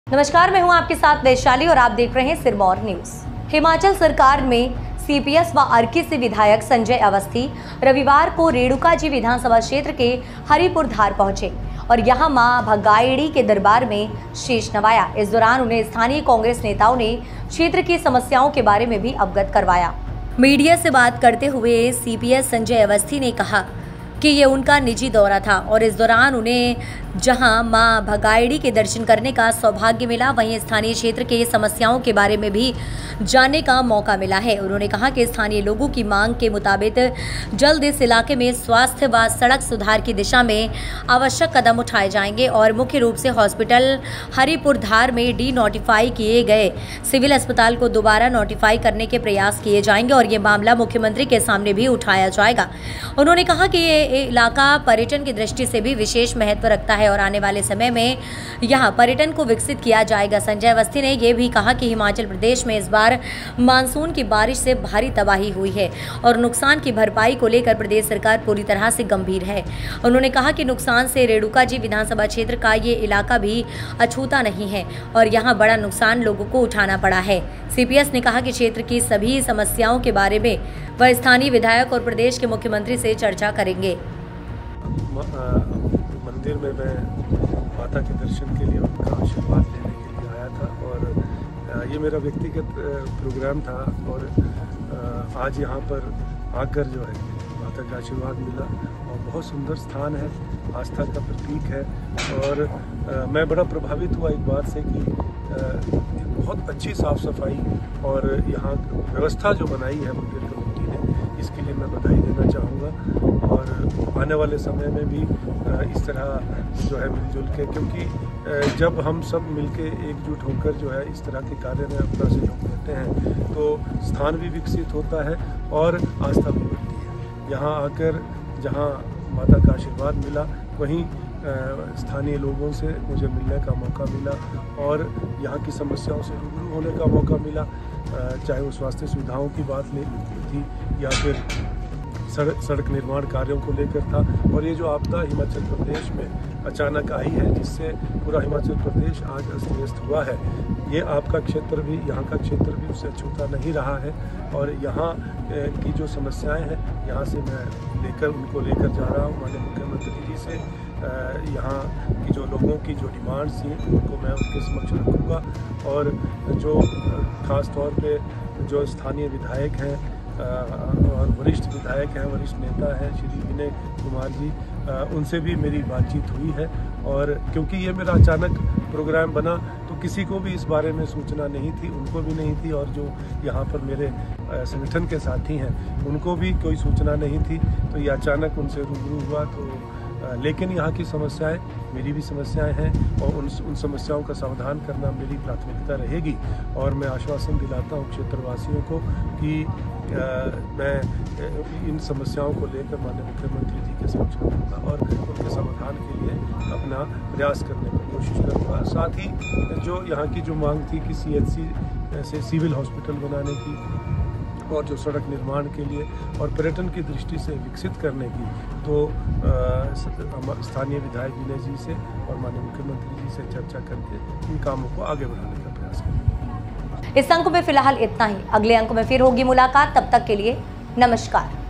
CPS अवस्थी बोले हरिपुरधार क्षेत्र में स्वास्थ्य सेवाओं में होंगा सुधार
हरिपुरधार में सिविल हॉस्पिटल करवाएंगे नोटिफाई:CPS अवस्थी